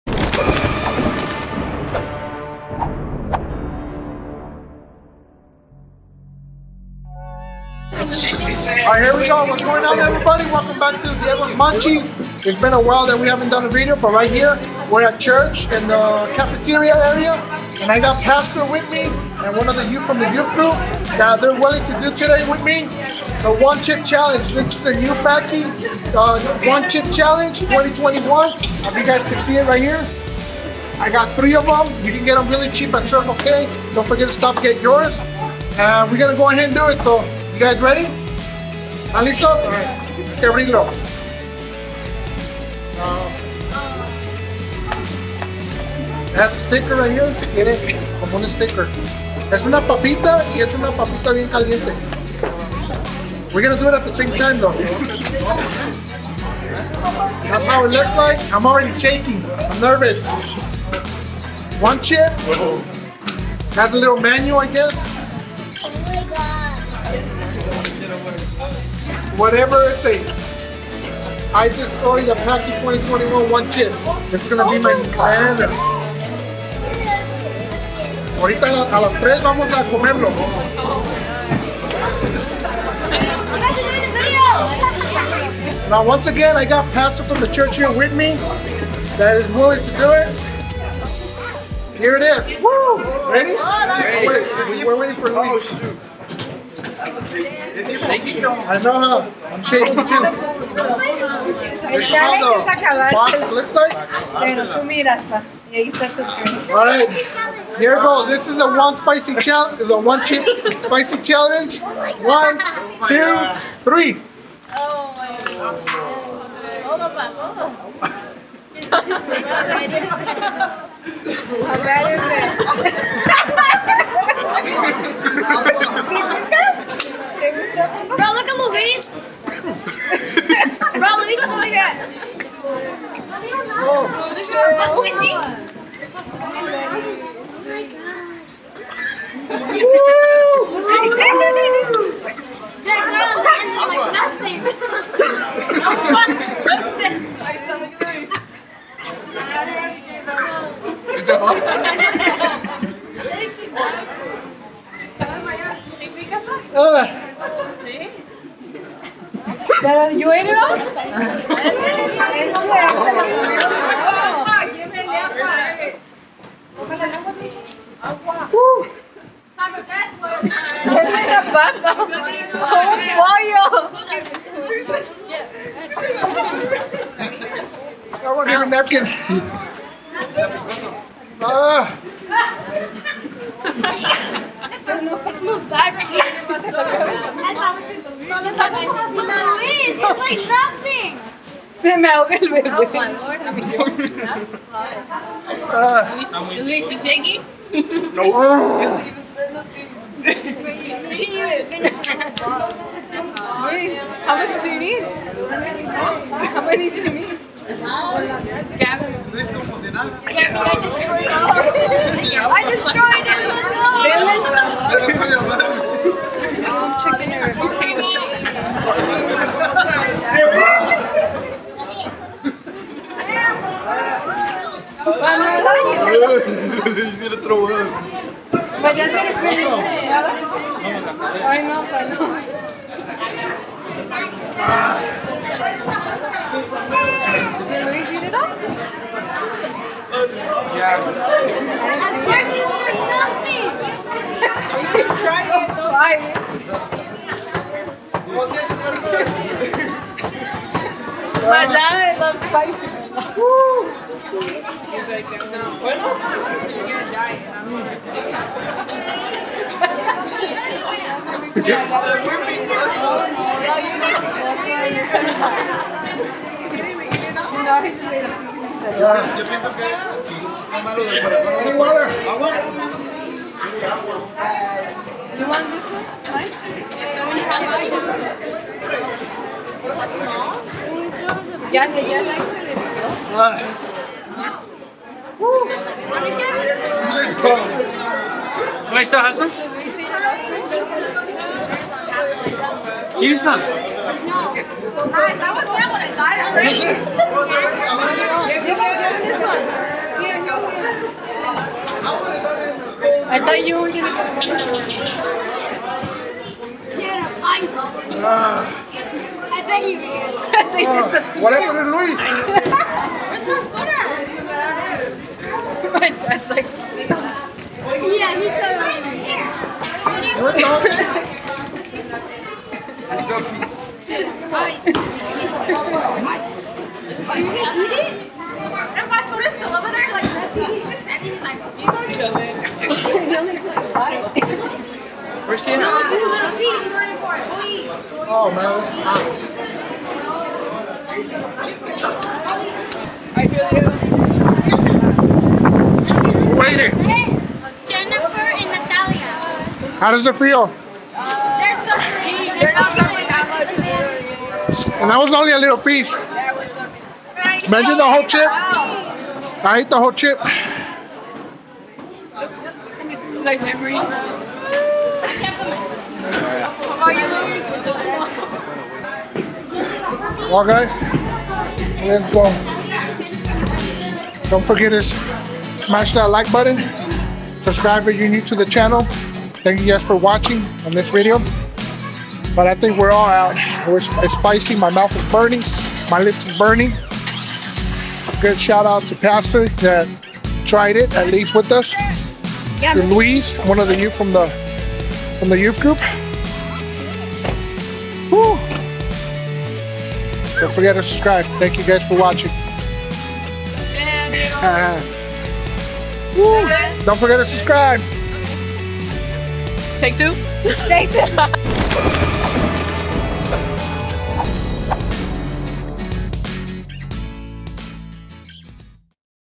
Paqui One Chip | 2021 Challenge | Full Video
This chip was the hottest chip iv'e ever tasted before. I want to give thanks to our Pastor from the church MinisteriosElReyEterno and the youth that wanted to participate with me.. Hope you guys like the video.